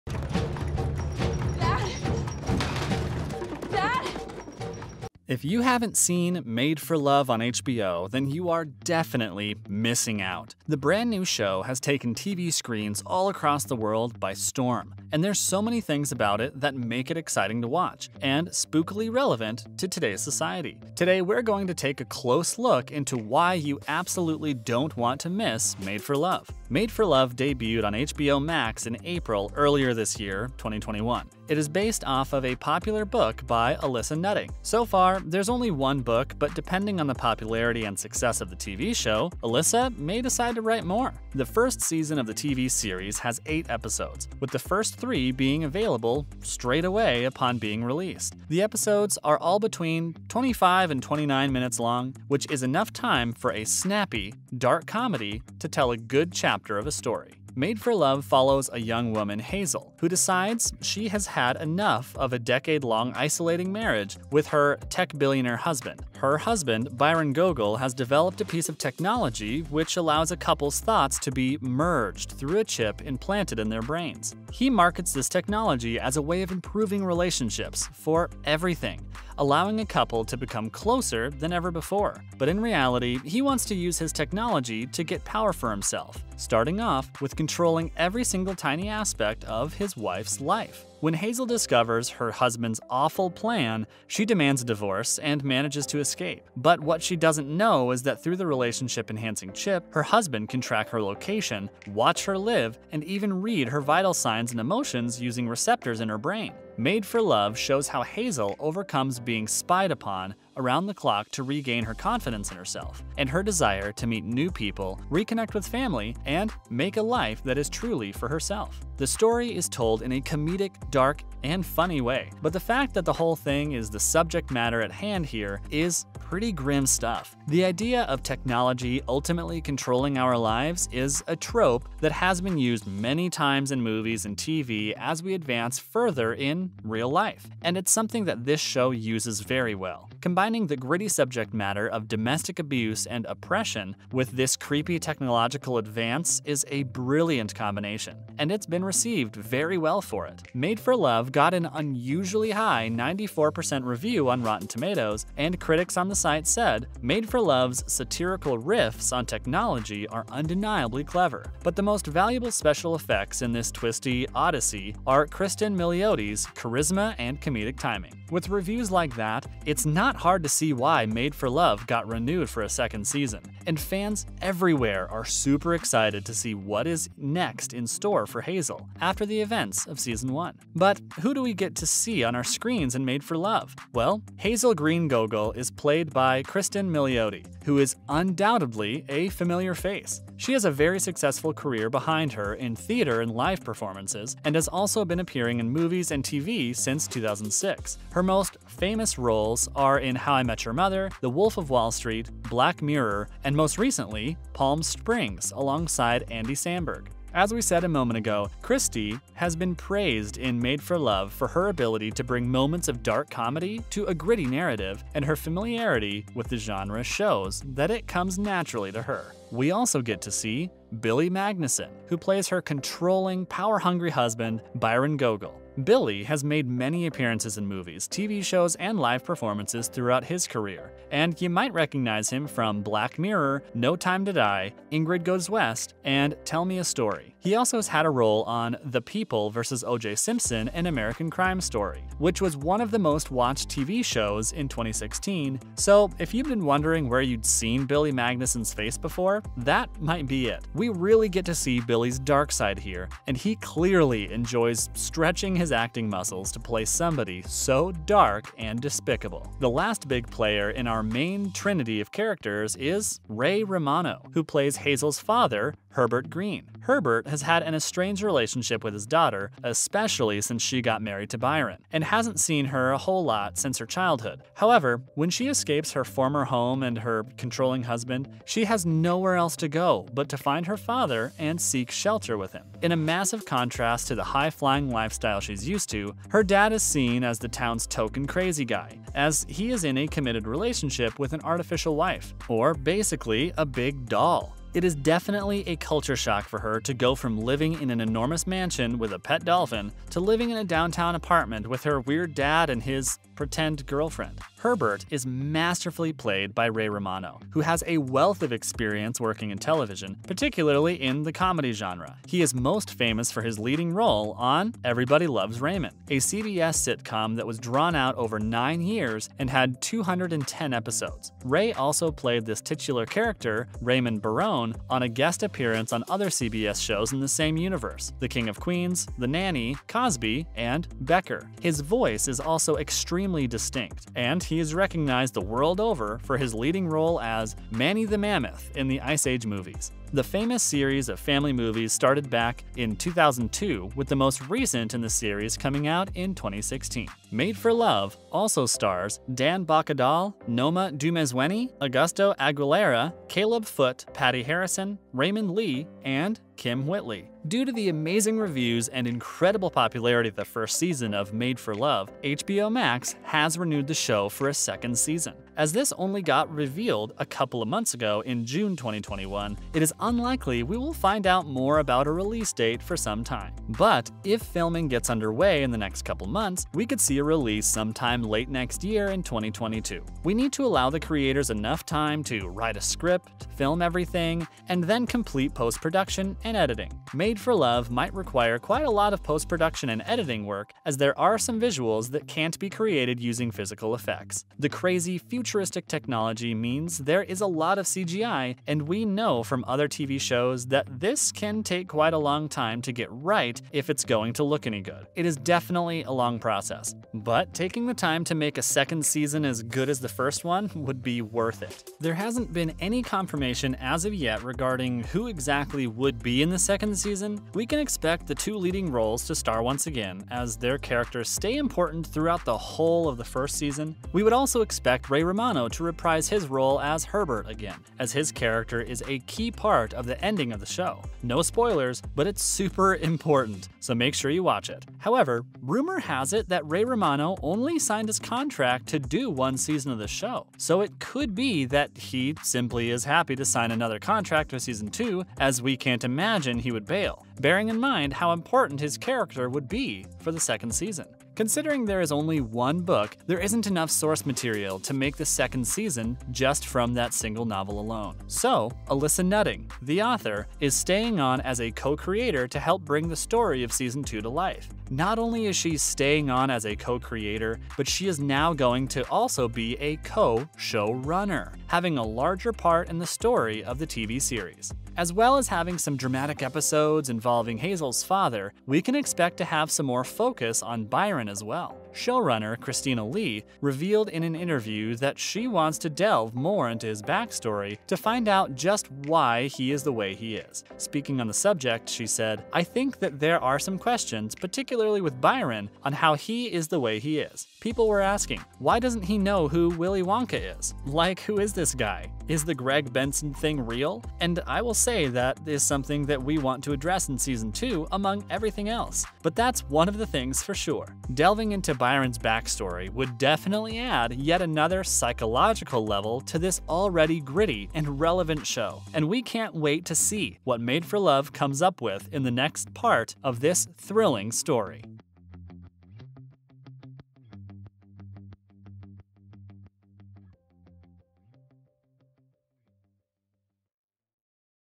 Made For Love Is A NEW Must Watch Show.. Here's Why!
Welcome back to Film Craze. Today on the channel, we will be featuring Made For Love is a new show you don't want to miss.. Here's why! Made For Love is a comedy, drama, sci-fi series from HBOMax Originals. Starring Cristin Milioti, Billy Magnussen, and Ray Romano.

From creators Dean Bakopoulos, Christina Lee, and Allison Nutting. Made For Love follows the story of a young woman who found out that her tech mogul husband has implanted a tracking device on her brain. Stick around and watch this video featuring Made For Love is a new show you don't want to miss.. Here's why!